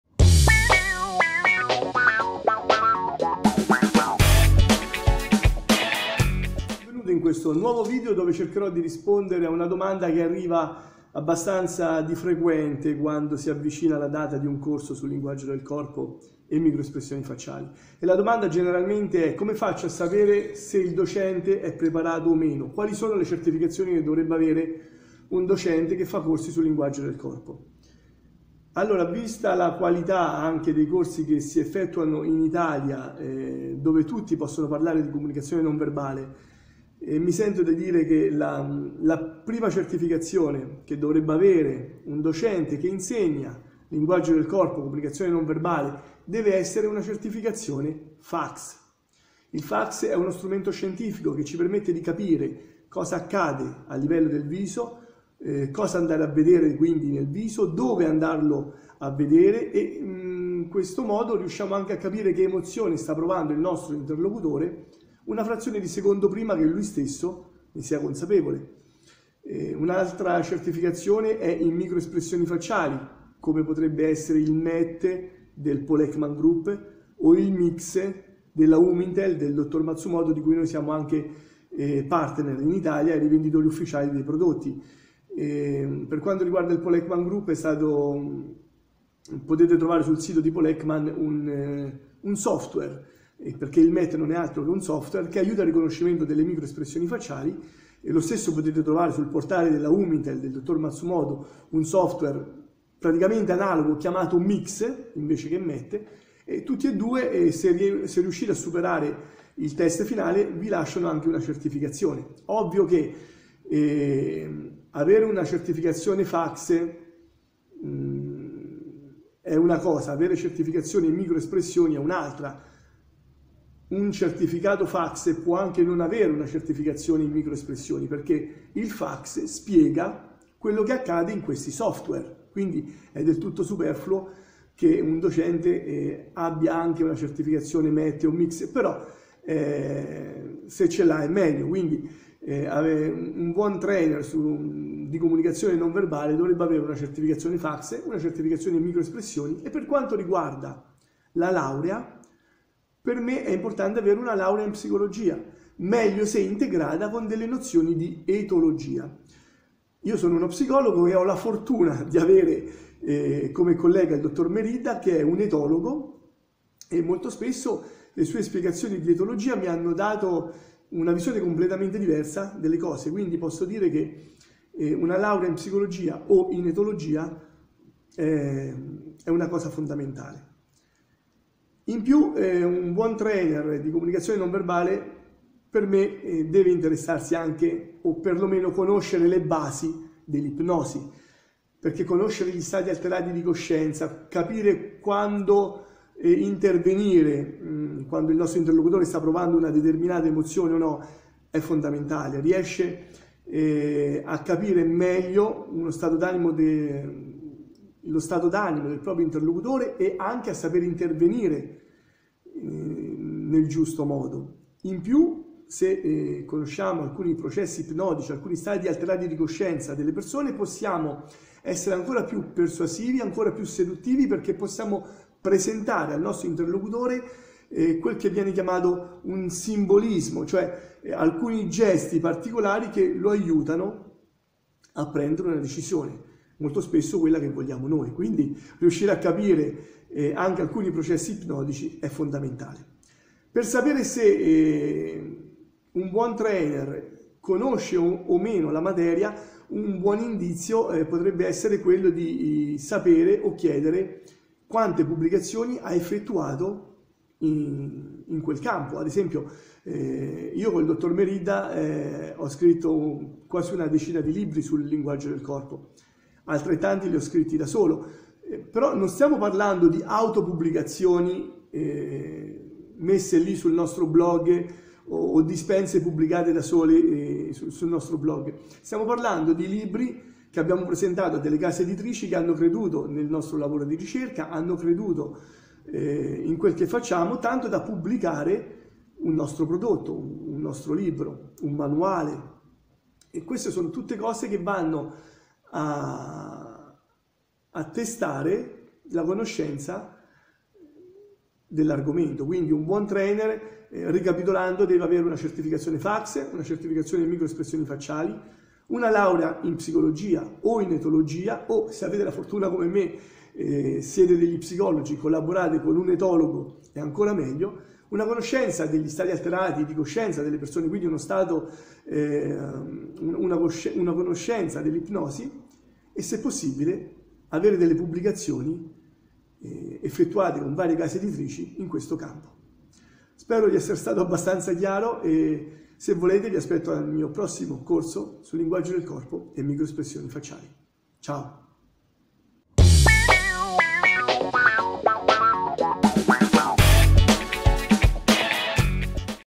Linguaggio del corpo. Come scegliere la propria formazione
Se stai cercando un metodo o dei docenti preparati, come puoi fidarti di loro?

Come puoi esser certo di riuscire ad imparare veramente il linguaggio del corpo e le micro espressioni facciali?

Il mondo della formazione in Italia, è sempre stato (a parte qualche eccezione) di livello infimo se non proprio ingannevole.

Non voglio stare qui a dirti che i corsi che facciamo noi sono i migliori, è come dire all’oste se il suo vino è buono, voglio solo elencarti le caratteristiche base che una buona scuola o un buon docente DEVE necessariamente avere.

Formae Mentis di Fabio Pandiscia - Linguaggio del corpo e PNL